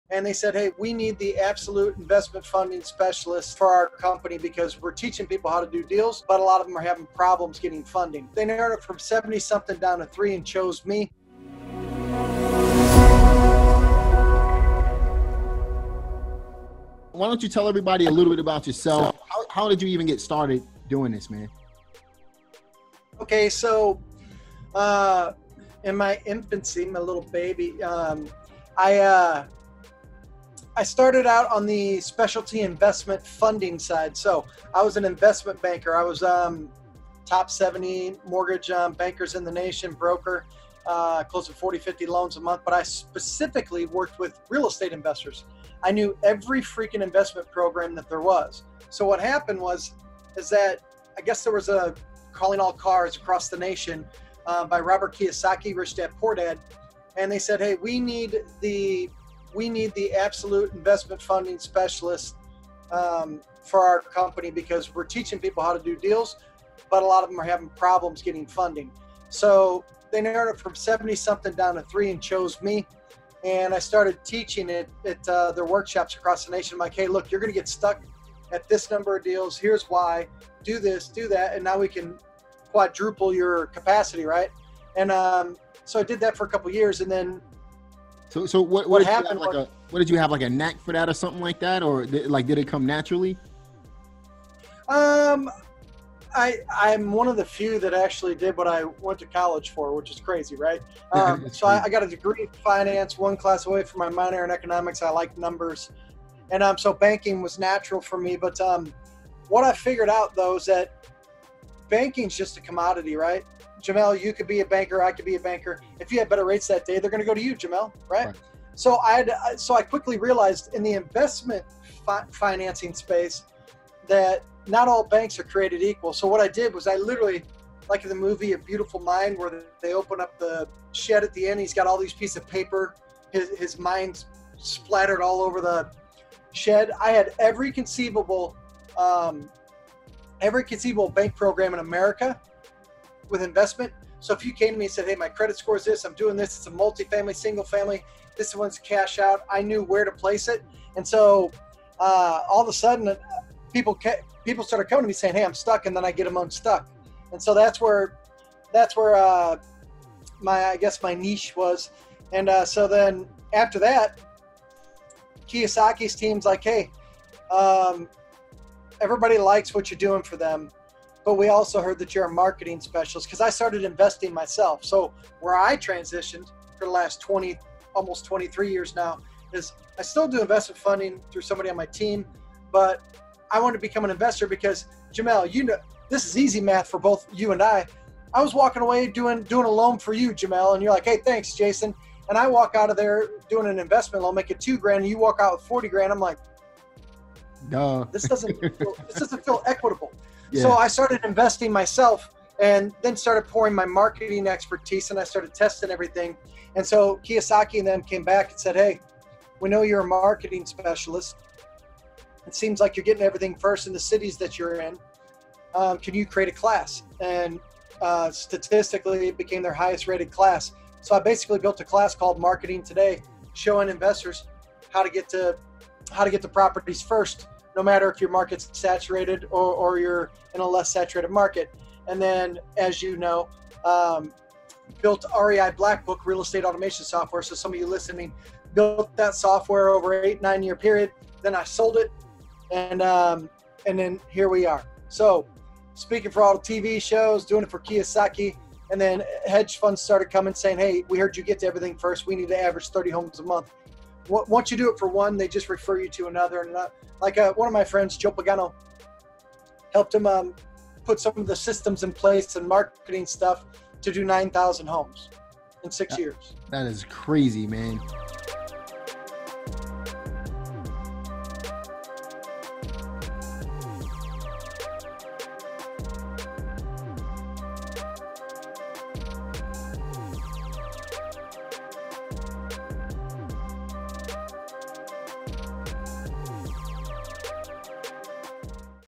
How I Started Learning Different Real Estate Investing Strategies
The blueprint we give real estate investors is a result of years of experience in finance and real estate investing – experience that I had from when I was working with real estate investors until I became one myself. These methods have been used not just by successful investors, but even companies as big as Robert Kiyosaki’s!

Watch now to learn how I started learning different real estate investing strategies and what the journey was like for me to get to where I am today!

00:00 Introduction
00:51 How did I get the opportunity to work with Robert Kiyosaki’s team?
01:30 Where did I learn about banking and finances?
02:43 Why did I decide to become an investor too?
03:27 How did all my experience help me to get to where I am today?